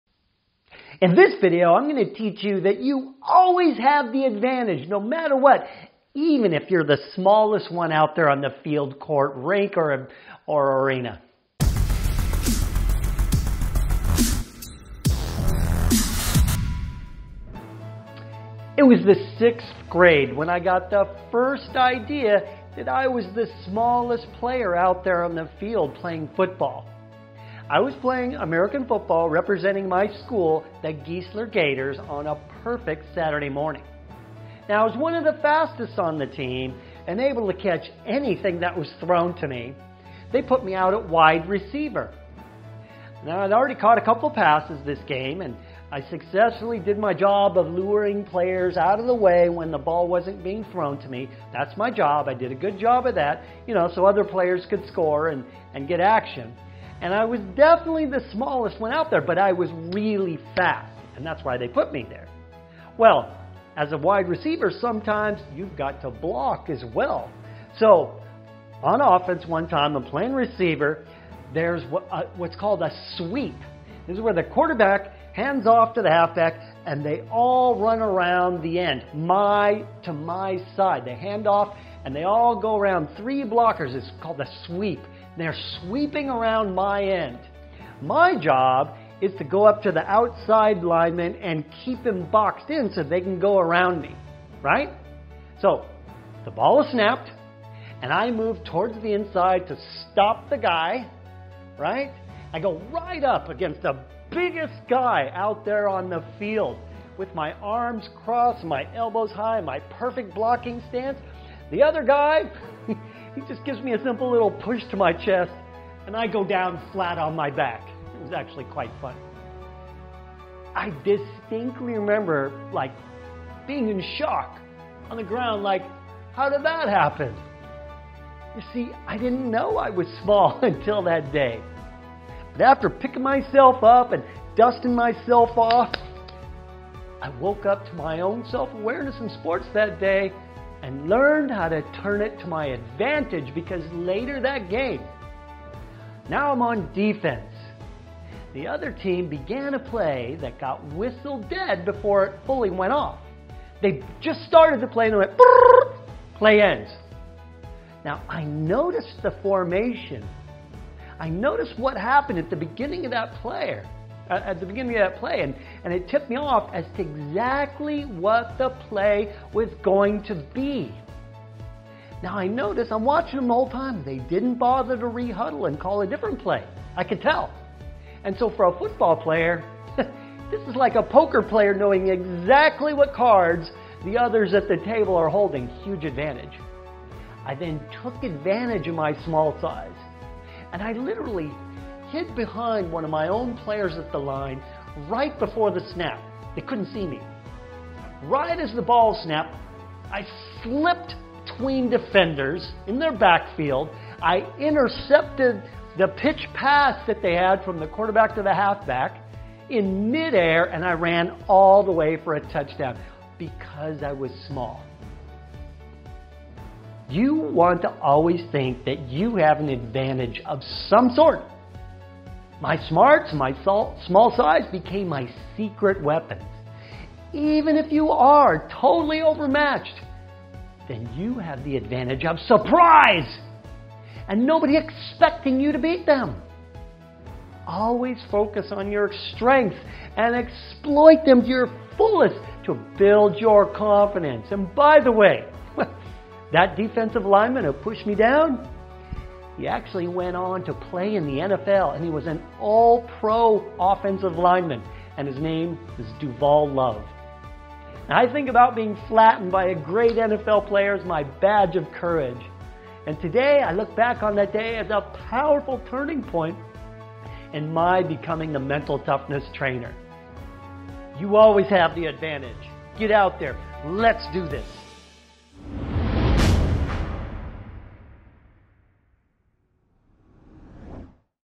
Mental Toughness: How To Turn "Weakness" Into The Best Advantage
Is it possible for you to create CONFIDENCE on command?

No one is perfect, this saying is probably the most cliche ever but it holds such true and deep meaning to it. In sports, everyone has their own insecurity, even Michael Jordan or Tiger Woods have definitely insecurities that once hindered them. What's their secret? They just simply turned their insecurities/weakness into their greatest advantage. This video will tell you the things you need to hear to start believing in your greatest advantage ever!